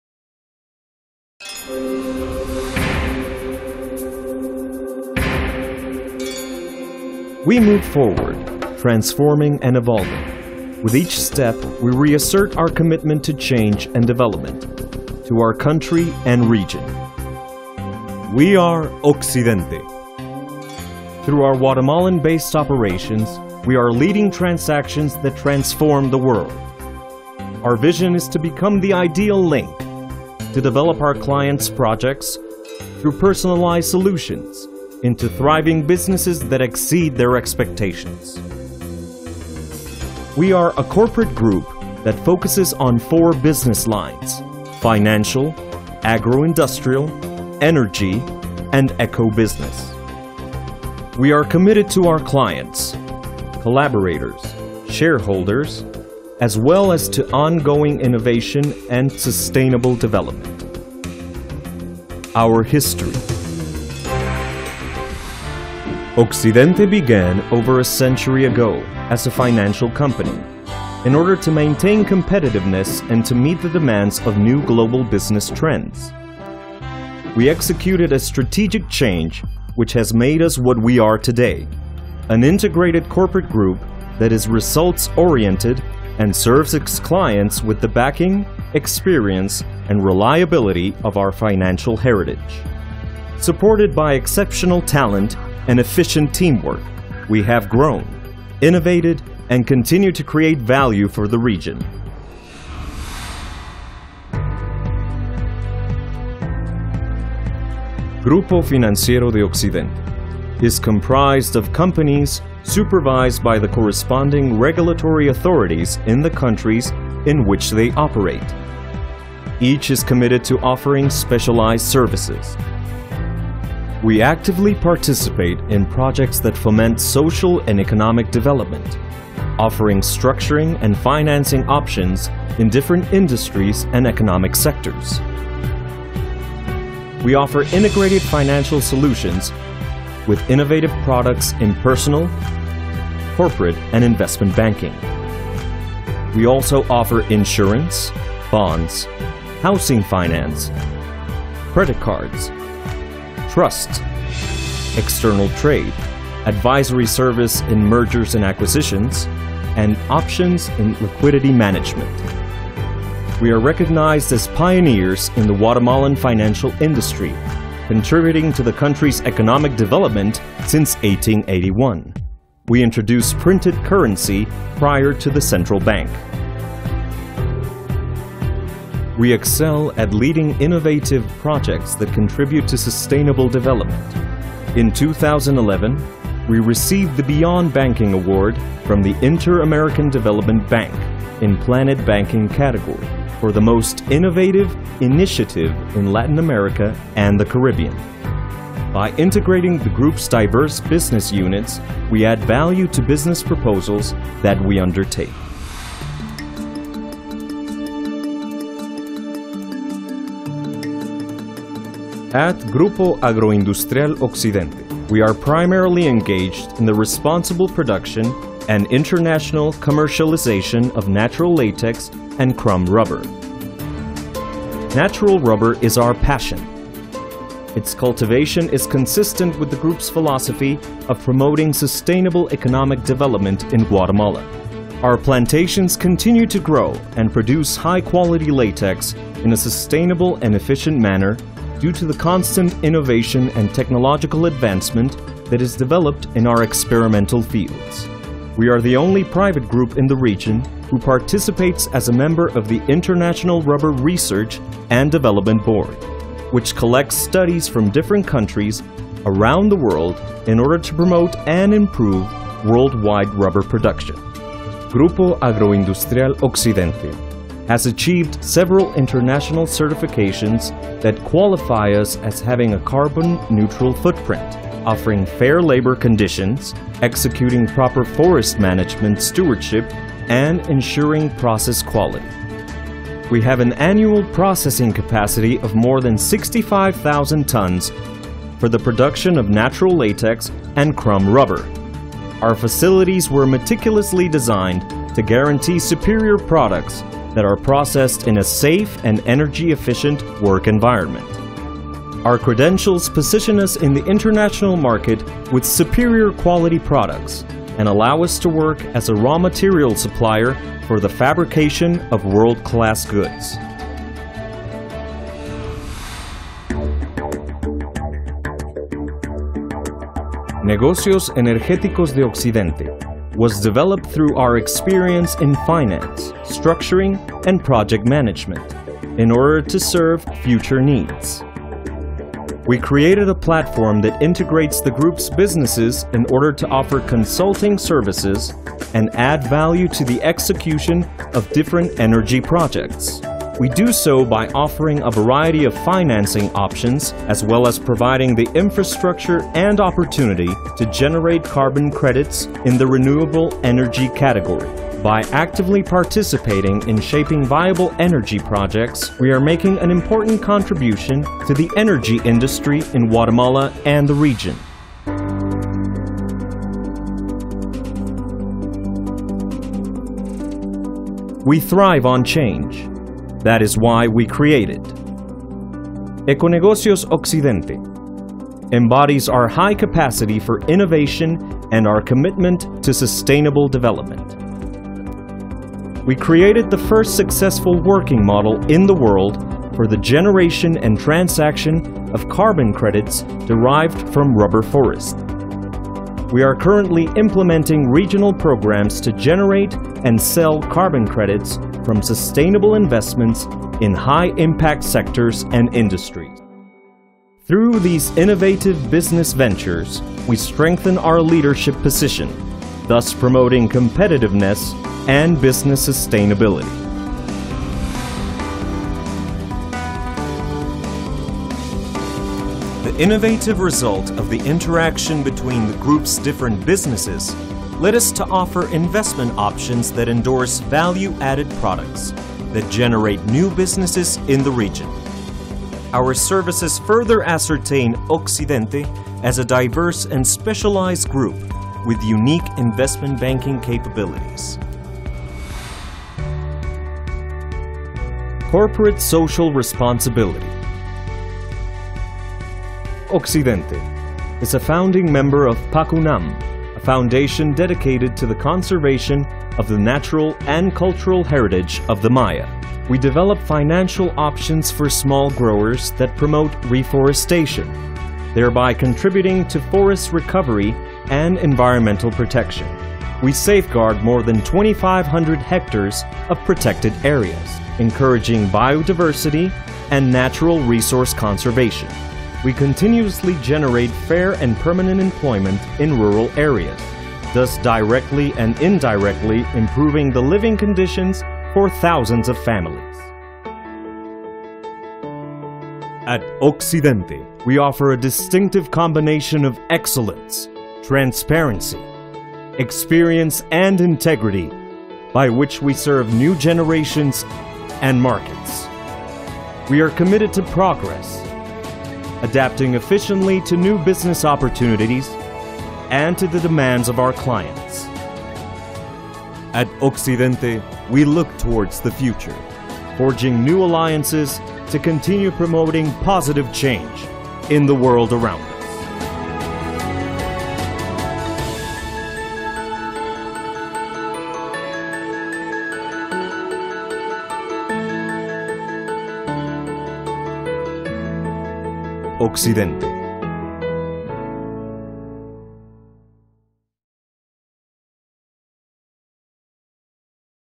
Institutional Video Grupo Occidente
Grupo Occidente is a dinamic company composed of 4 lines of business: finance, agroindustrial, energy and ecobusiness.